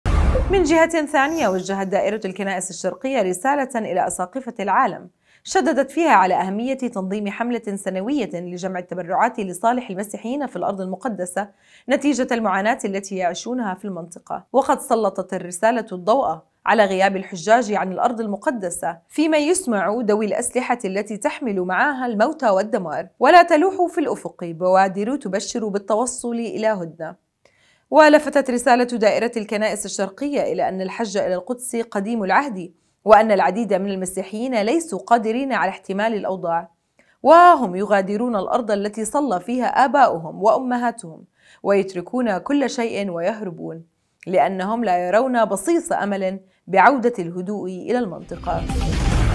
وجّهت دائرةُ الكنائسِ الشرقيه رسالةً إلى أساقفةِ العالم
من جهةٍ ثانيه وجّهت دائرةُ الكنائسِ الشرقيه رسالةً إلى أساقفةِ العالم / شدّدت فيها على أهميةِ تنظيمِ حملةٍ سنويةٍ لجمعِ التبرعاتِ لصالحِ المسيحيينَ في الأرضِ المقدسه نتيجةَ المعاناةِ التي يعيشونها في المنطقه / وقد سلطت الرسالةُ الضوءَ على غيابِ الحجاجِ عن الأرضِ المقدسة فيما يُسمعُ دويُ الأسلحةِ التي تحملُ معها الموتَ والدمار / ولا تلوحُ في الأفقِ بوادرُ تبشرُ بالتوصلِ إلى هدنه / ولفتت رسالةُ دائرةِ الكنائسِ الشرقيه إلى أنَّ الحجَ إلى القدسِ قديمُ العهدِ وأنَّ العديدَ من المسيحيين ليسوا قادرينَ على احتمالِ الأوضاع / وها هم يغادرونَ الأرضَ التي صلّى فيها آباؤُهم وأمهاتُهم ويتركونَ كلّ شيءٍ ويهربون / لأنهم لا يرونَ بصيصَ أملٍ بعودةِ الهدوءِ الى المنطقه